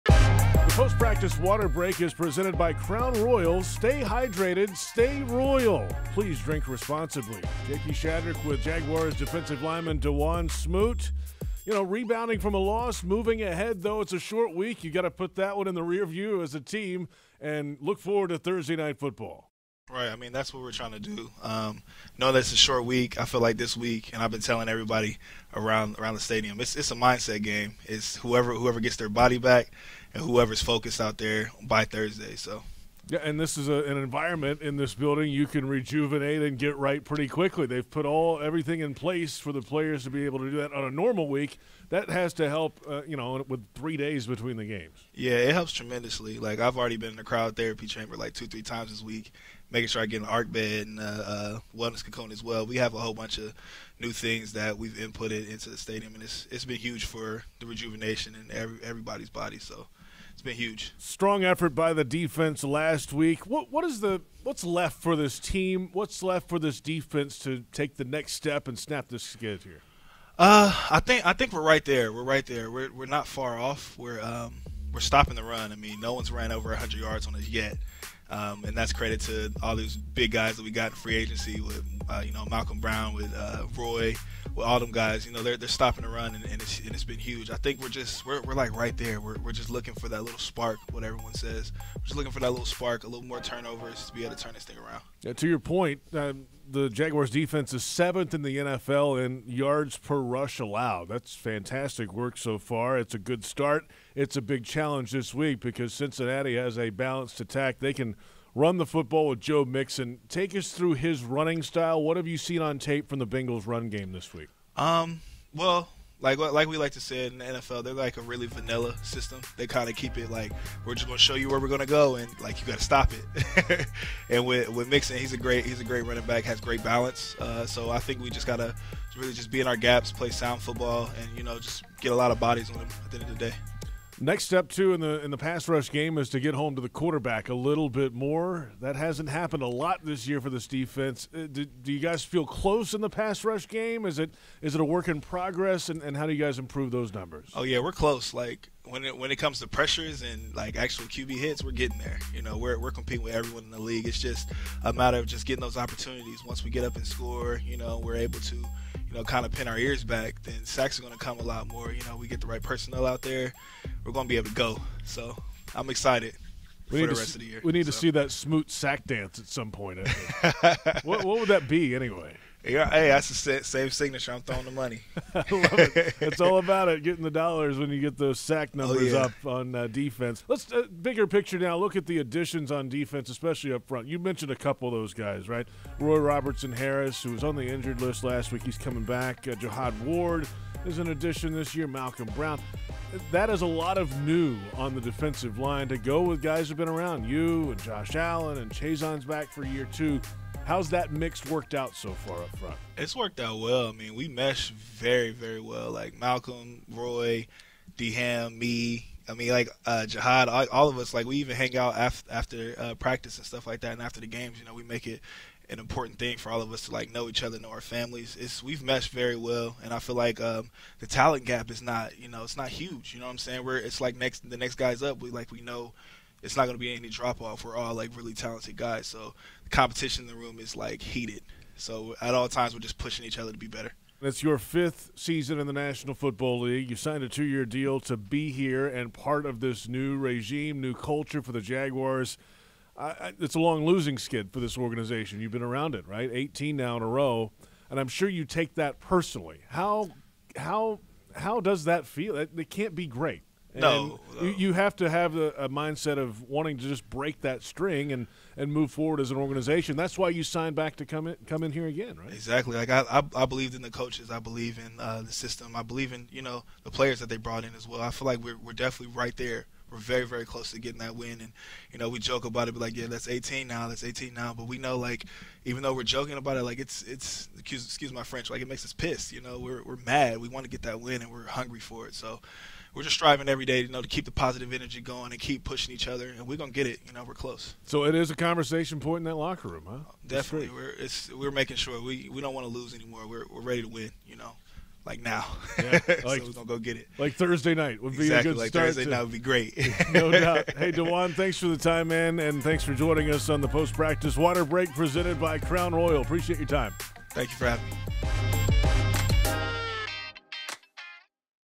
Crown Royal Water Break: Dawuane Smoot
J.P. Shadrick catches up with defensive end Dawuane Smoot after practice for the Crown Royal Water Break.